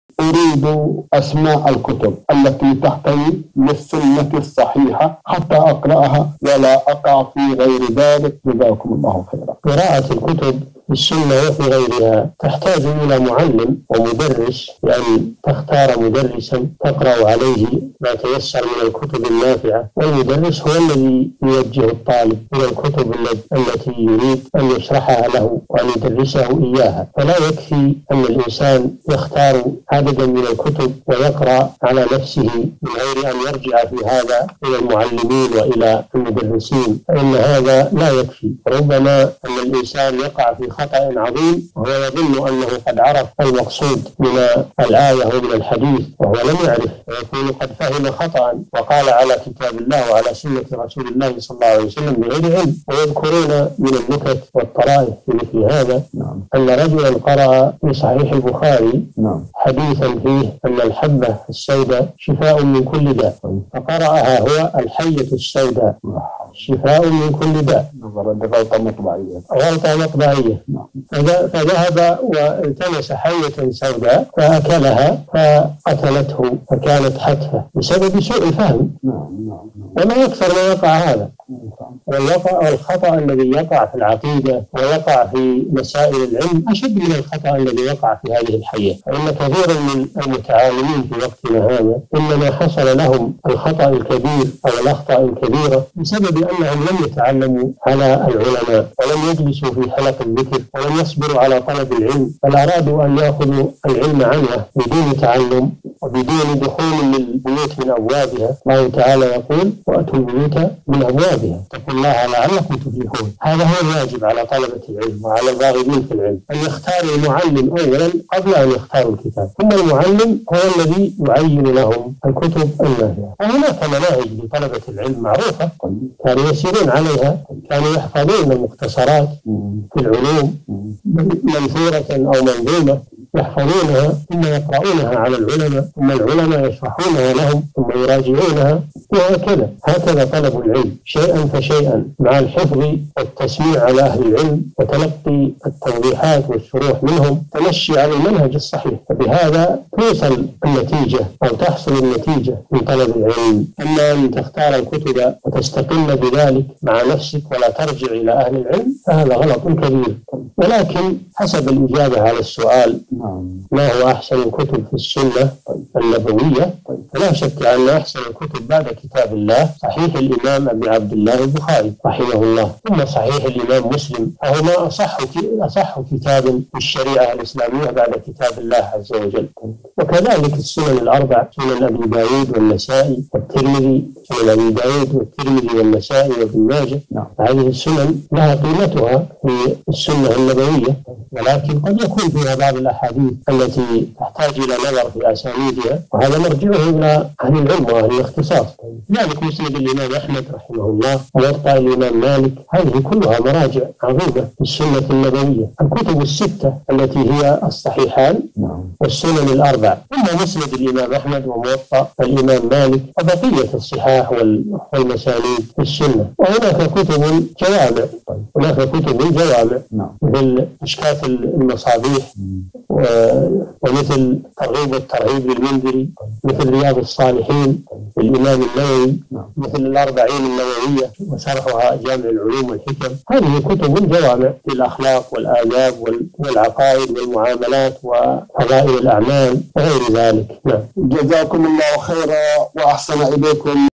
أريد أسماء الكتب التي تحتوي للسنة الصحيحة حتى أقرأها ولا أقع في غير ذلك؟ الشيخ صالح بن فوزان القوزان
نص السؤال:
أريد أسماء الكتب التي تحتوي للسنة الصحيحة حتى أقرأها ولا أقع في غير ذلك جزاكم الله خيراً؟
السيرة الذاتية :
للاشتراك في قناة الشيخ صالح الفوزان

رئيس الفريق
لمشروع كبار العلماء